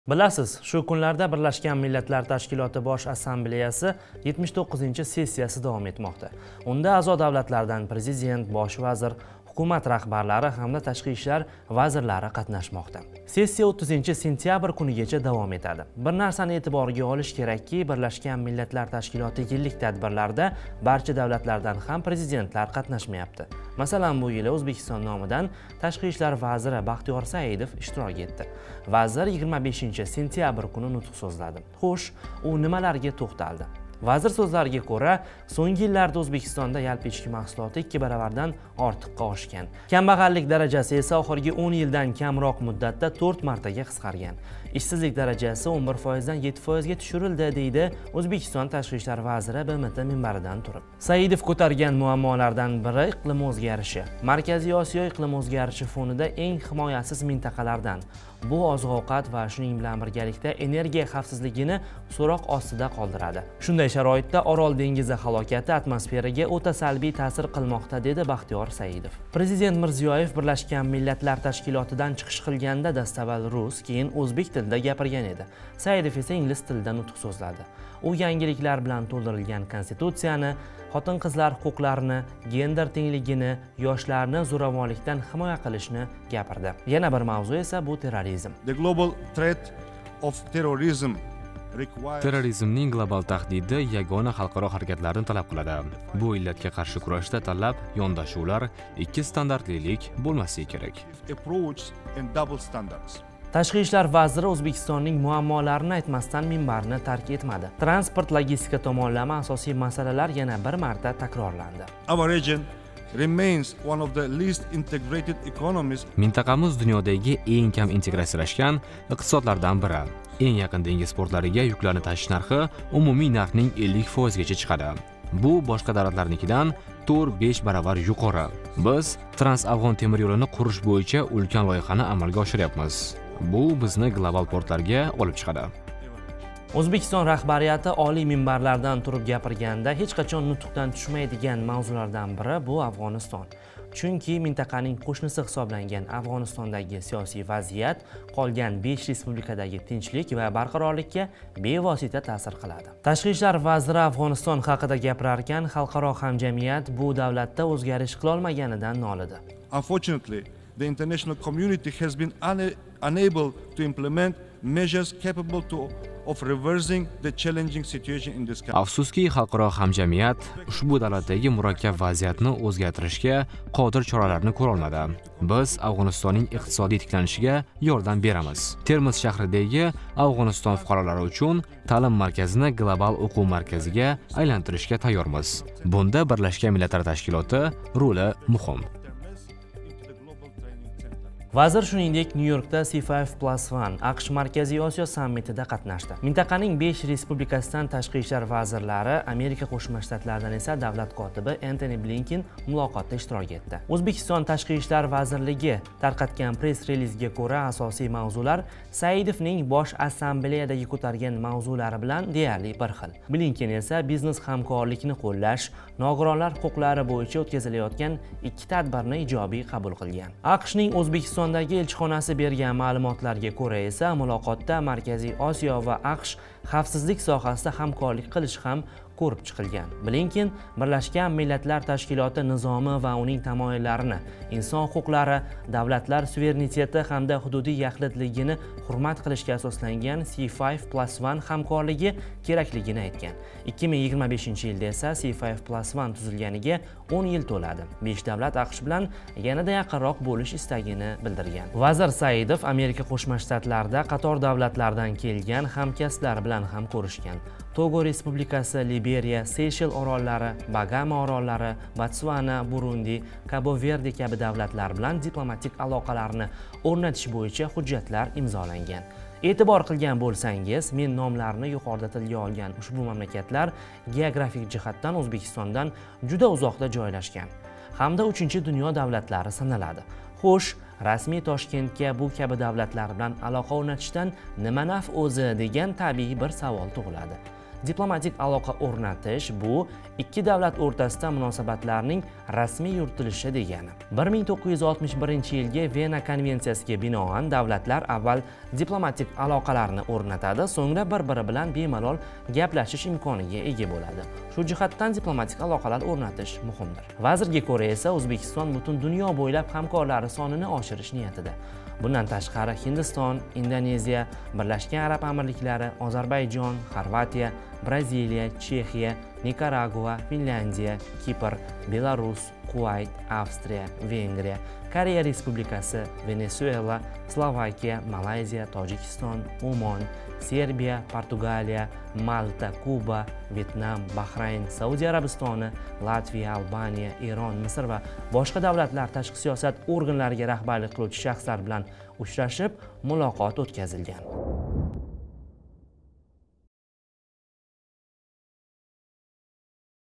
Muammolar, transport-logistika va Afg‘oniston - Saidov BMTda
Shu kunlarda BMTning Bosh assambleyasi 79-sessiyasi davom etmoqda. Unda O‘zbekiston nomidan Tashqi ishlar vaziri Baxtiyor Saidov ishtirok etdi va nutq so‘zladi. Kun.uz Saidov BMT minbarida e'tibor qaratgan masalalarga to‘xtaldi.